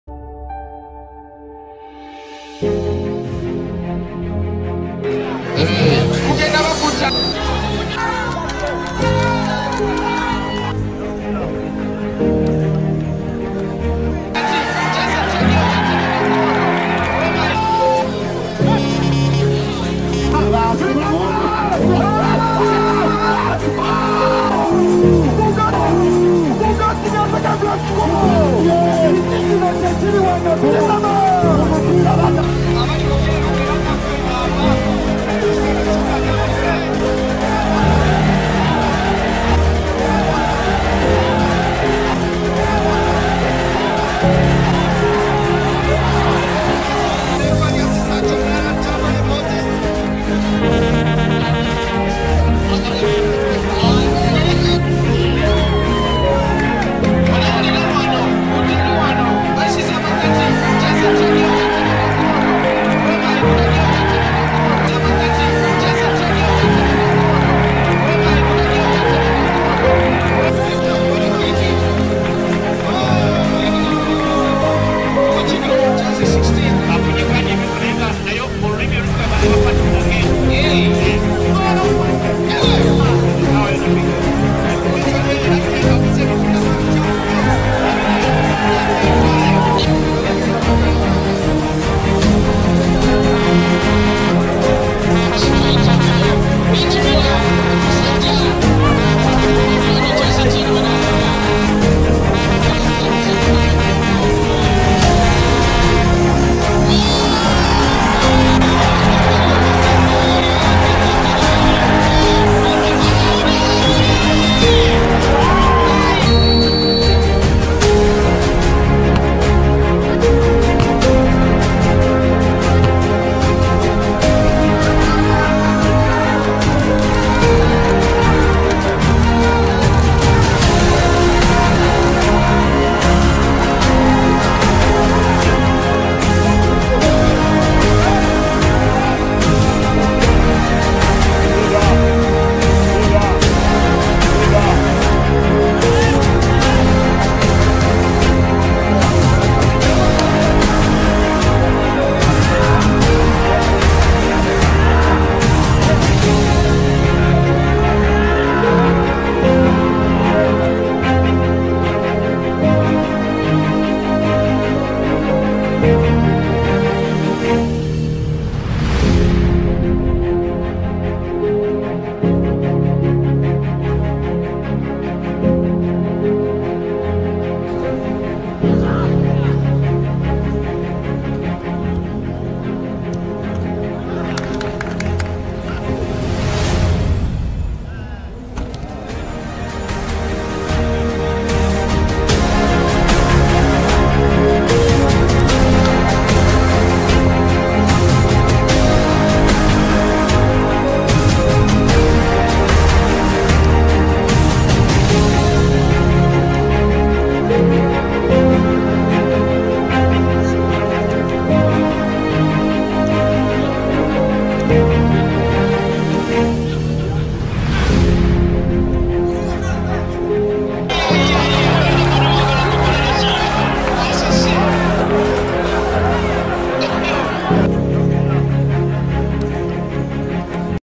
Joseph Kizza Bukenya. Skills and trickery with KCCA FC Junior team and Buddo SS team.
Joseph Kizza Bukenya former Buddo Joseph Kizza Bukenya former Buddo SS school team player and former KCCA FC junior team player.
Now a member of KCCA FC senior team.